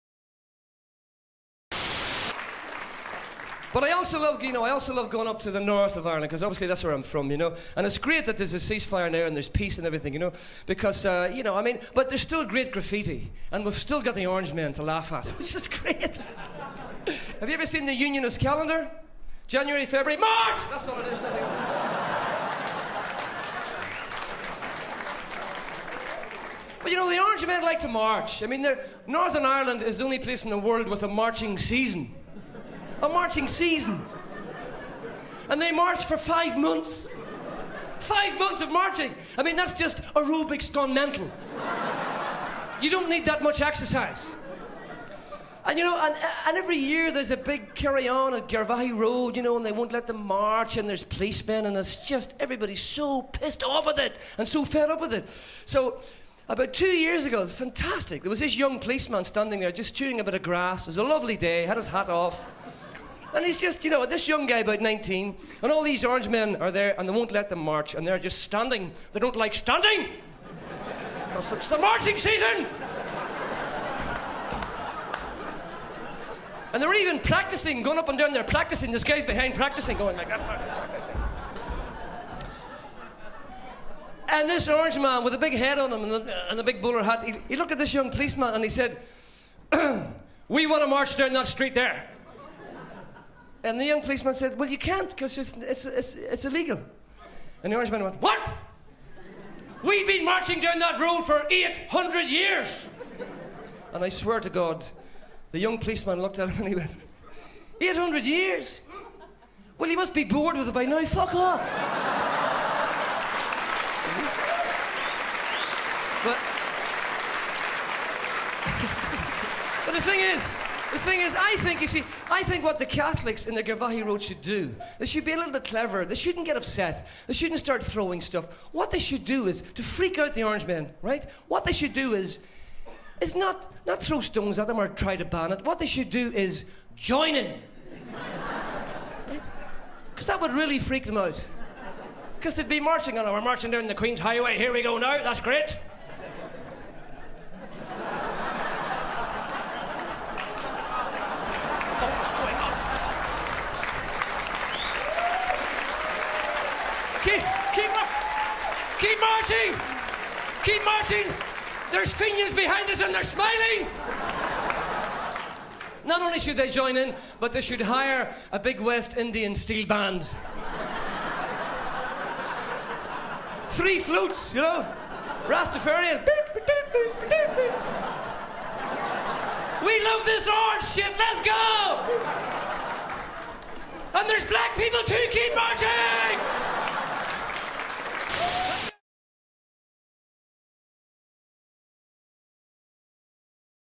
Owen O'Neill on the Orange March | Grintage Ireland
Owen O'Neill talks about the Orange March at the Liffey Laughs.